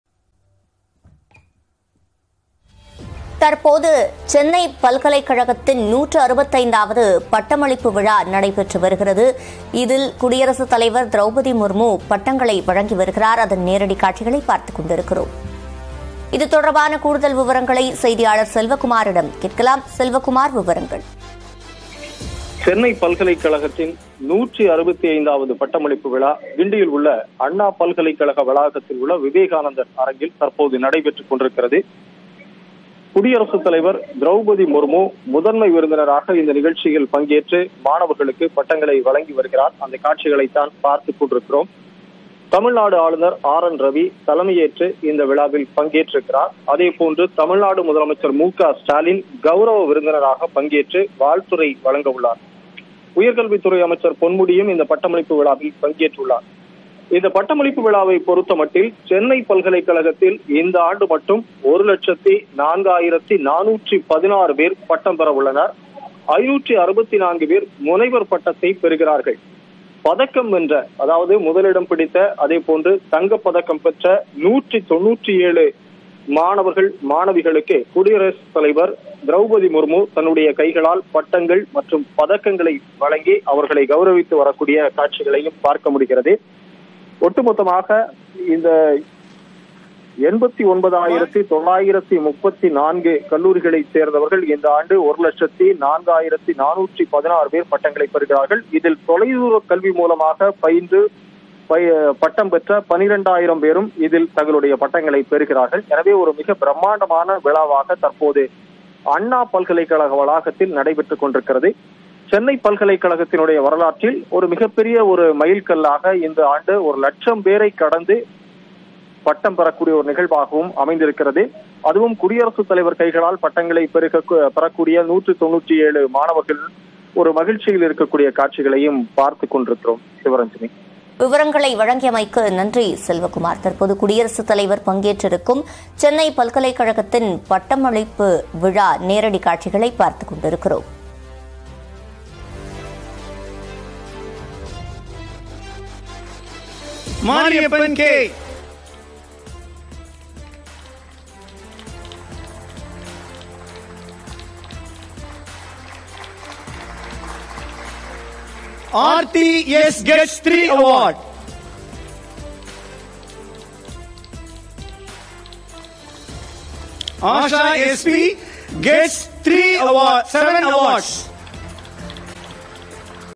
Anna University | சென்னை பல்கலை பட்டமளிப்பு விழா - குடியரசுத் தலைவர் பங்கேற்பு | Tamil News
சென்னை பல்கலை பட்டமளிப்பு விழா - குடியரசுத் தலைவர் பங்கேற்பு. செய்திகளை துல்லியமாகவும் துரிதமாகவும் வழங்கும் தமிழகத்தின் முதன்மை செய்தி தொலைக்காட்சி உங்கள் நியூஸ்18 தமிழ்நாடு.

News18TamilNadu | Tamil News | Anna University